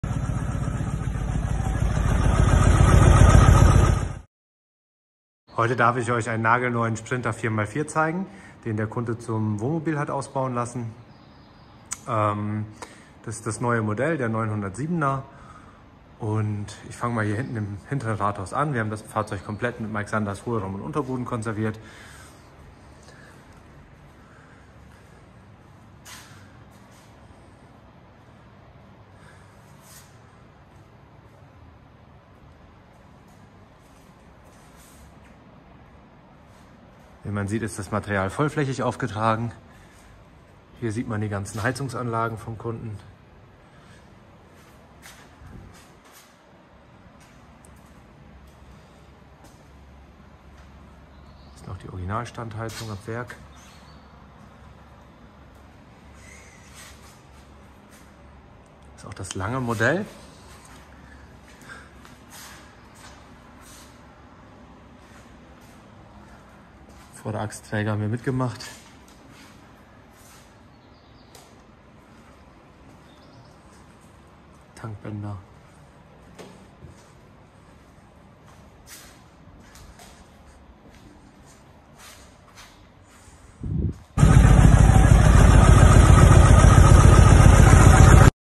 Mercedes Sprinter 4x4 907 906 Hohlraumversiegelung und Unterbodenschutz mit Mike Sanders Vanlife
In diesem Video zeigen wir Ihnen eine Hohlraumversiegelung & Unterbodenversiegelung (Unterbodenschutz) am Beispiel eines Mercedes Benz W907 Sprinter 4x4 mit dem Korrosionsschutzfett Mike Sanders Fett. Warum Hohlraumversiegelung? How to do?